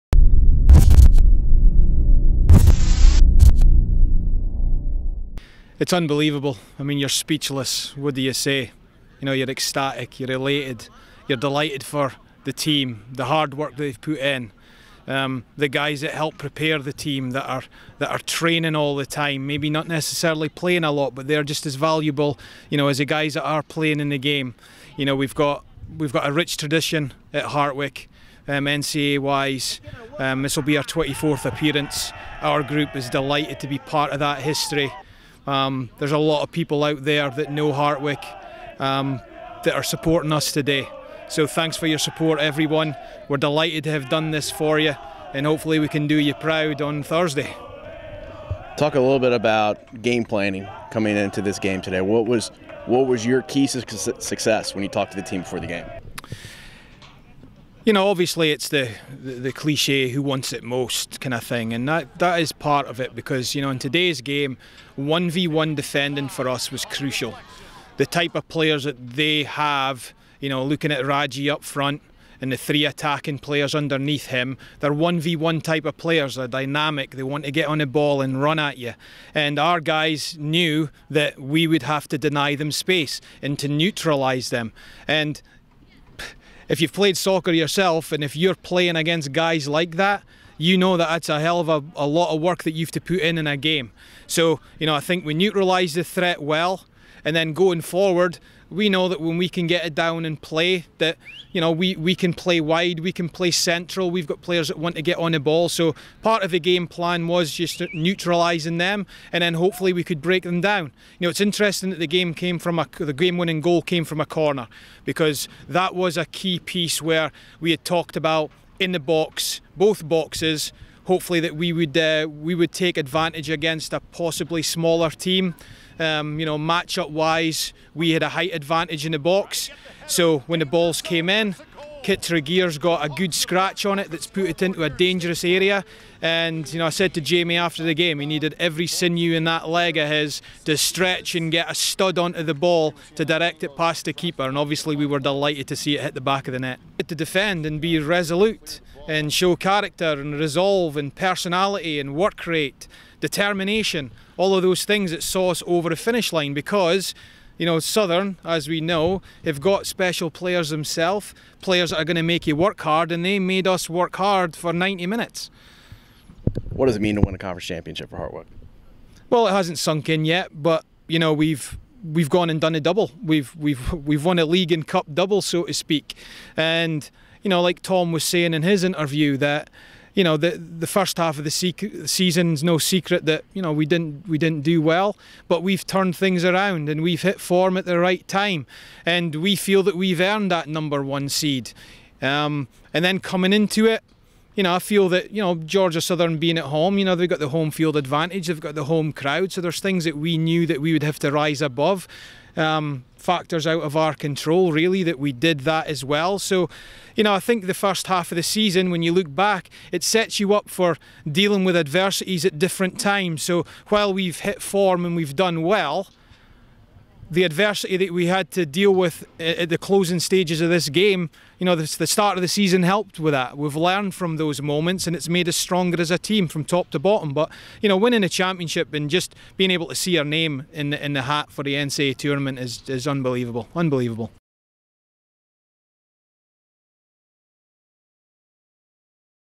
2014 Men's Soccer Championship Press Conference: Hartwick Head Coach John Scott
Hartwick Head Coach John Scott reacts to winning the 2014 Sun Belt Conference Men's Soccer Championship.